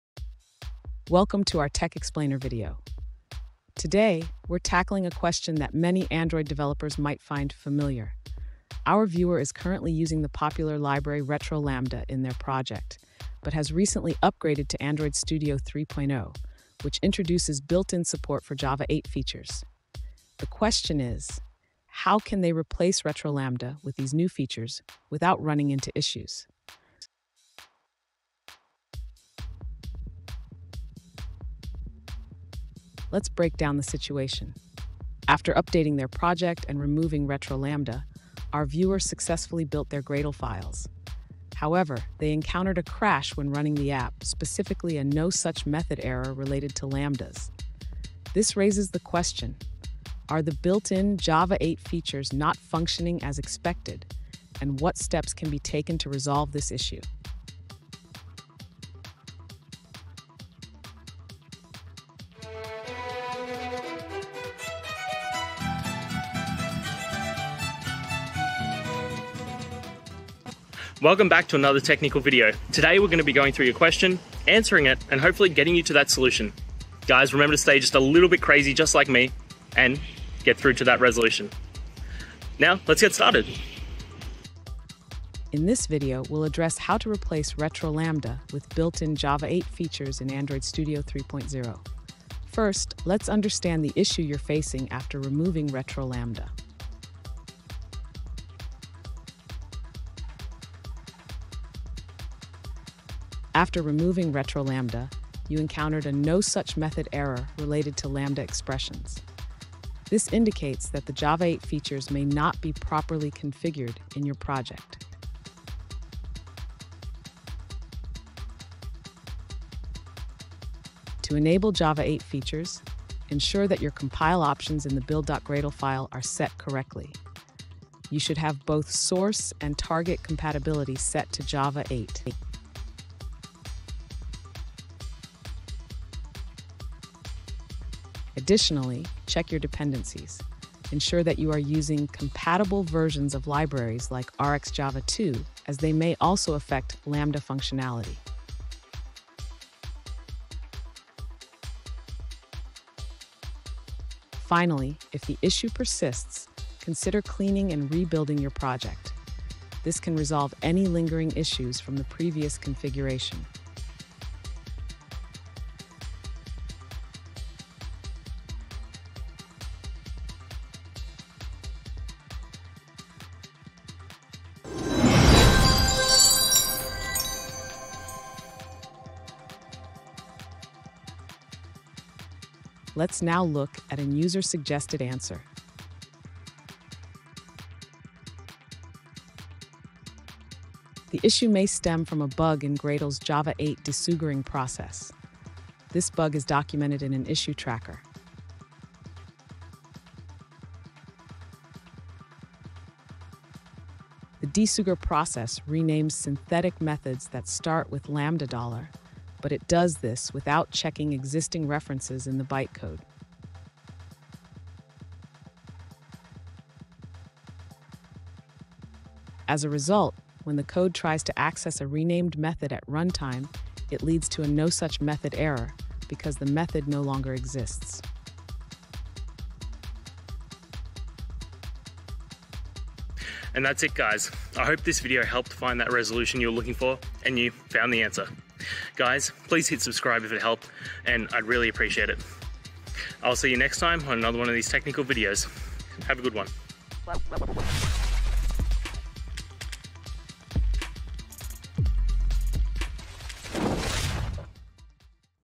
How to Replace Retrolambda with Java 8 Features in Android Studio 3.0
In this video, we’ll explore the transition from Retrolambda to the powerful features introduced in Java 8, specifically tailored for Android development in Android Studio 3.0. As we dive into the benefits of using Java 8's lambda expressions and streams, you'll learn how to modernize your codebase, improve readability, and enhance performance. Join us as we guide you through the step-by-step process of making this important upgrade in your Android projects.

Today's Topic: How to Replace Retrolambda with Java 8 Features in Android Studio 3.0

Related to